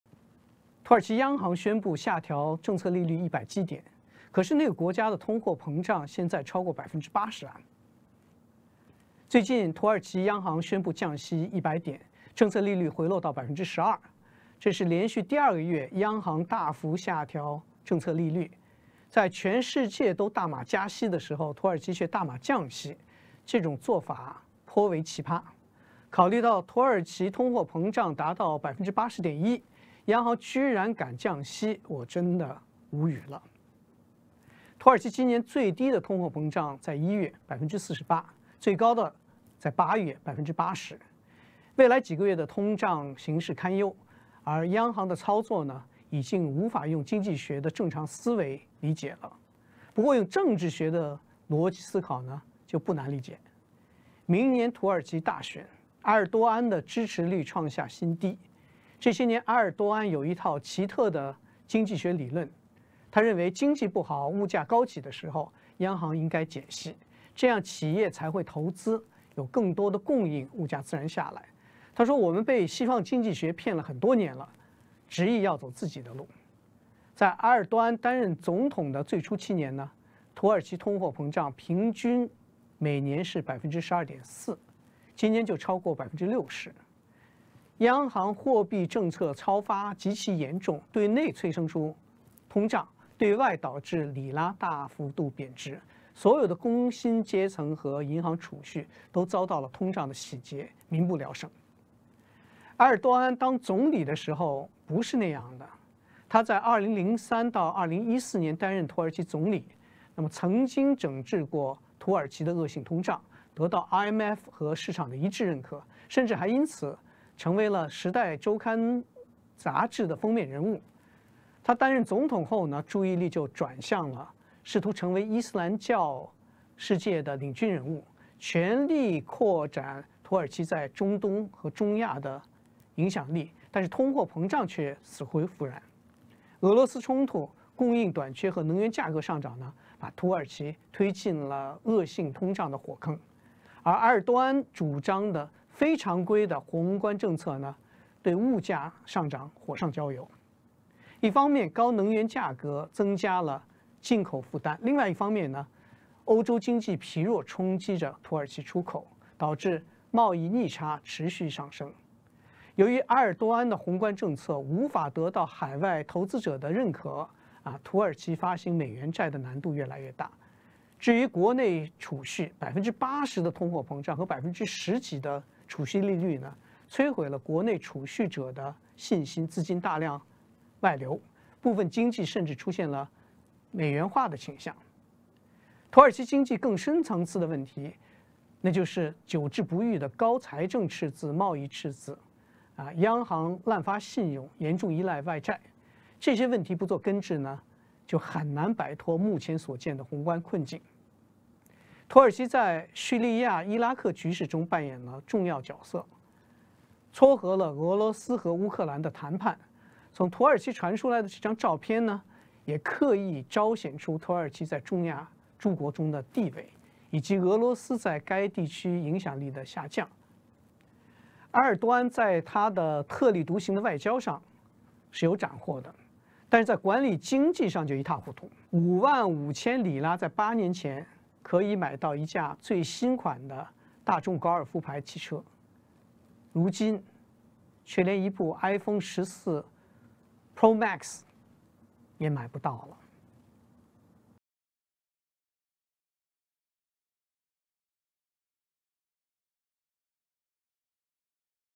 【專訪】陶冬：土耳其大碼減息應對通脹頗奇葩
5.5萬里拉在土耳其可以買到甚麼？8年前是一架最新款的大眾高爾夫牌汽車，如今卻連一部iPhone14 Pro Max也買不到。對於土耳其駭人的通脹，瑞信亞太區財富管理部大中華區副主席陶冬博士接受《經濟通通訊社》專訪時表示，土耳其總統埃爾多安的特立獨行外交頗有斬獲，但在管理經濟上就一塌糊塗。

　　他指，土耳其央行9月22日宣布下調政策利率100基點，政策利率回落到12%，是連續第二個月央行大幅下調政策利率，但這個國家的通脹現在超過80%。以減息應對高通脹，陶博直言做法頗為奇葩。央行的操作已經無法用經濟學的正常思維理解，不過用政治學的邏輯思考，就不難理解了——明年土耳其大選，埃爾多安的支持率創下新低。

etnet App 一版比較哂銀行匯率，睇完互動圖表再set埋預設提示，隨時把握最優匯價，超級方便！📱😄😄
【限時優惠$88/月】申請etnet強化版MQ手機串流報價服務